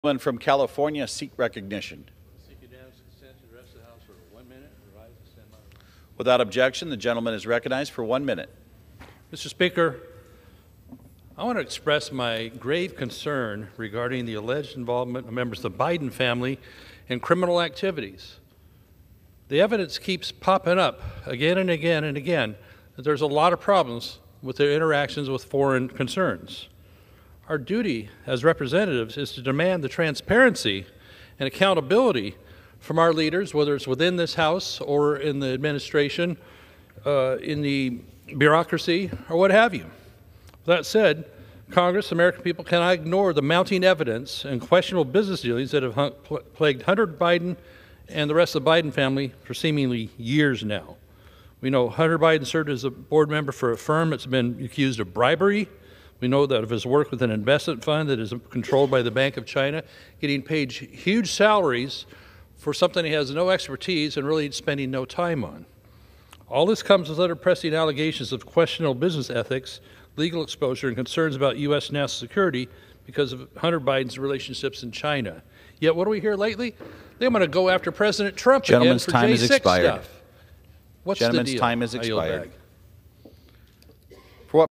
Doug LaMalfa Sounds Off On 'The Alleged Involvement... In Criminal Activities' By The Biden Family
Rep. Doug LaMalfa (R-CA) spoke about alleged criminal activity carried out by Hunter Biden and connections to the President himself.